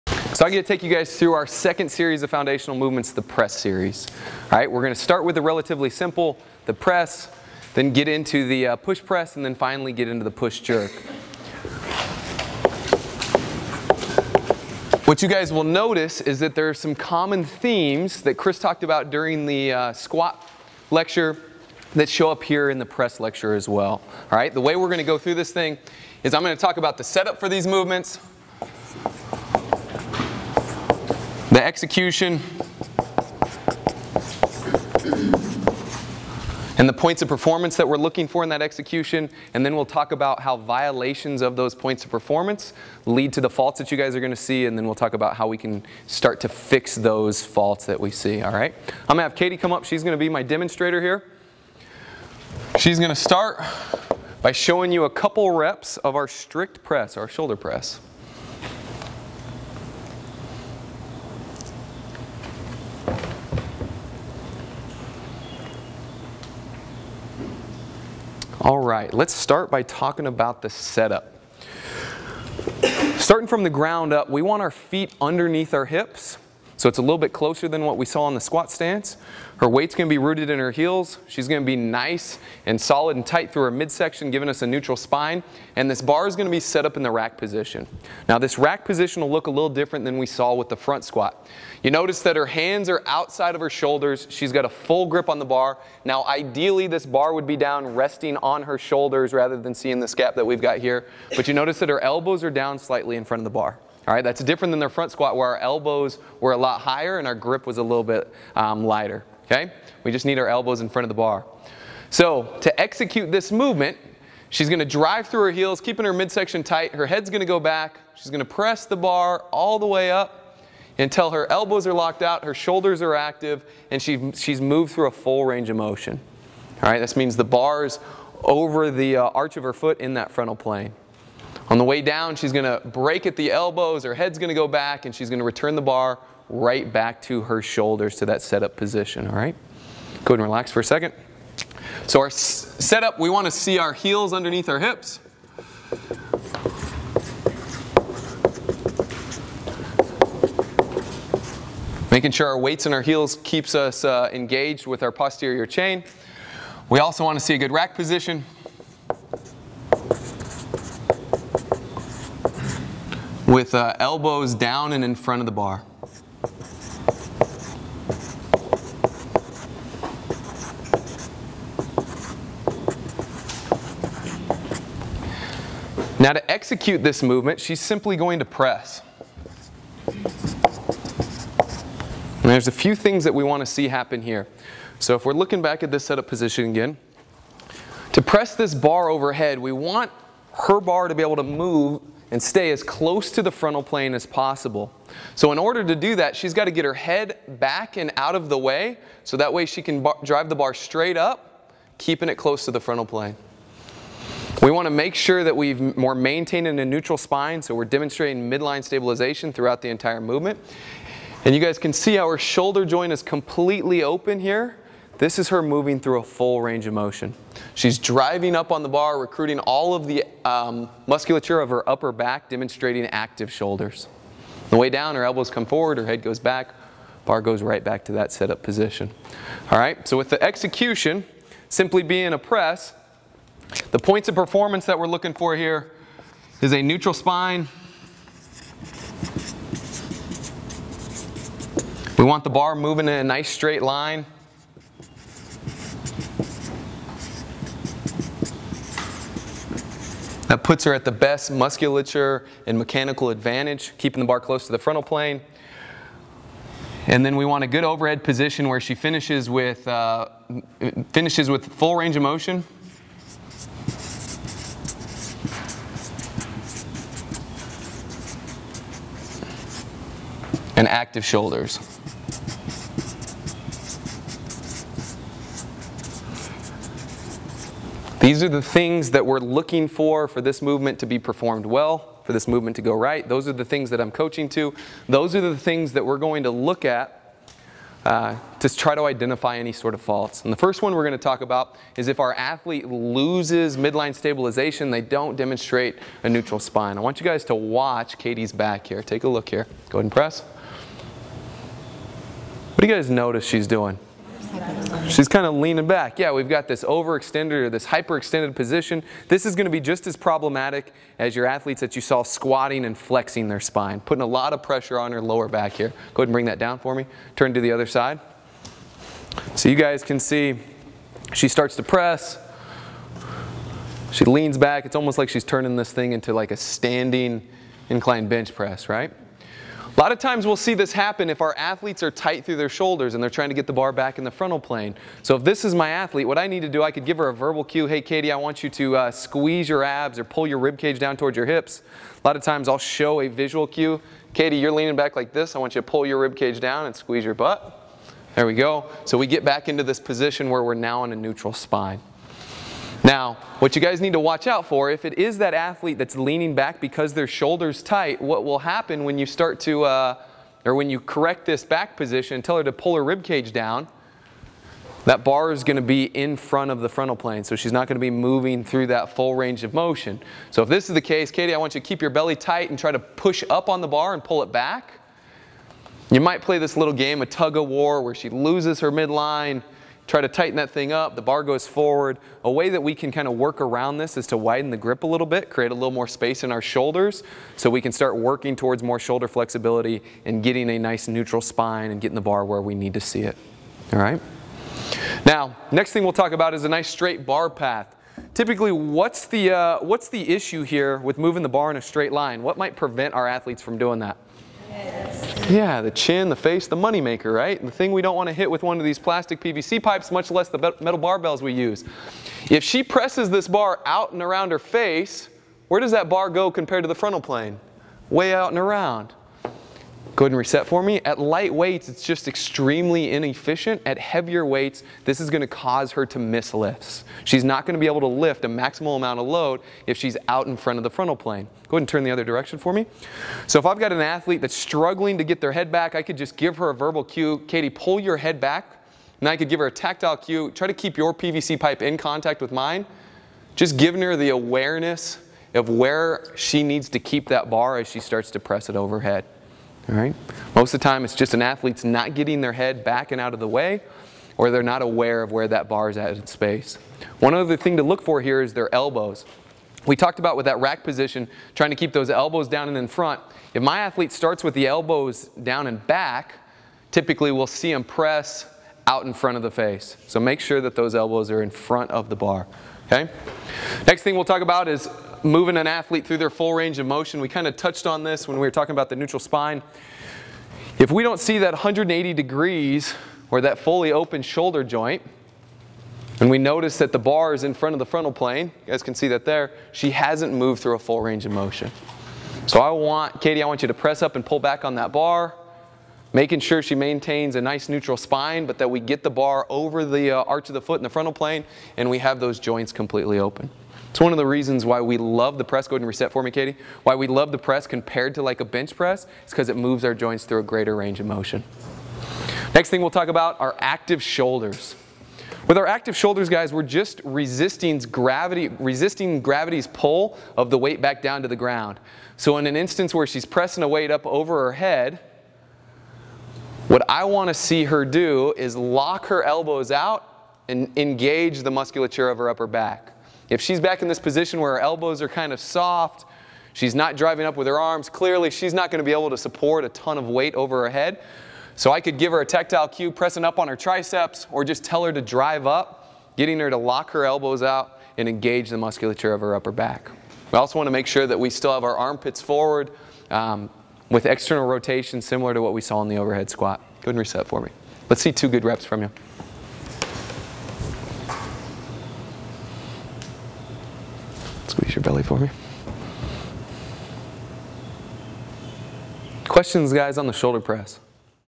The Presses, Part 1: The Strict Press
"These lifts are enormous aids to developing the power zone (hip flexors, hip extensors, spinal erectors, and quadriceps). As the athlete progresses through the lifts, the importance of core to extremity muscle recruitment is learned and reinforced. This is foundational to the effective and efficient performance of athletic movement, and alone would justify the practice and training of these lifts." (CrossFit Journal, 2003).